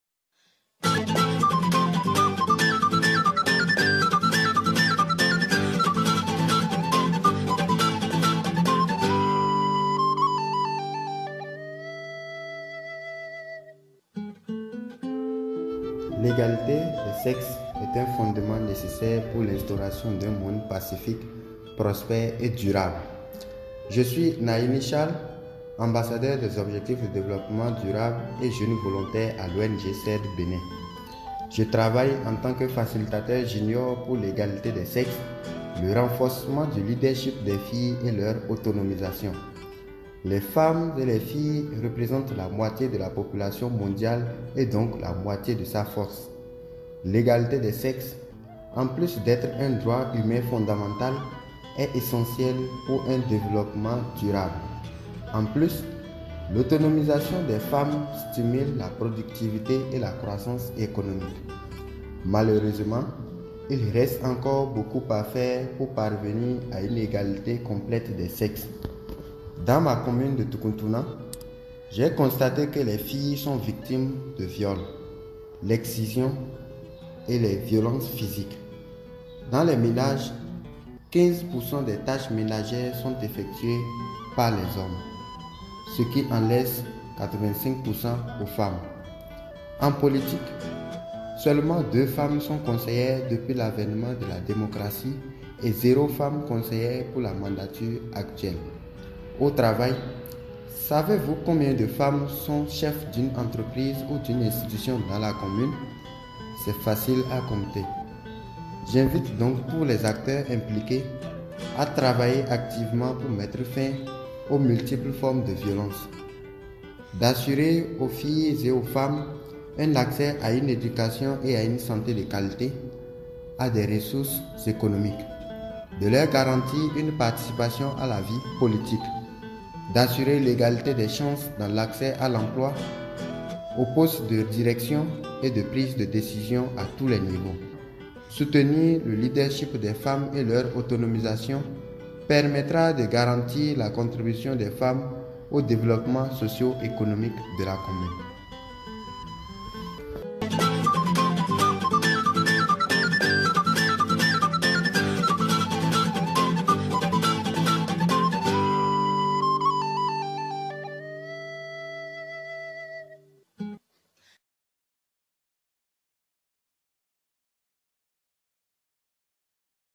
L’égalité des sexes est un fondement nécessaire
Les femmes et les filles représentent la moitié de la population mondiale et donc la moitié de sa force. Soutenir le leadership des femmes et leur autonomisation permettra de garantir la contribution des femmes au développement socio-économique de la commune.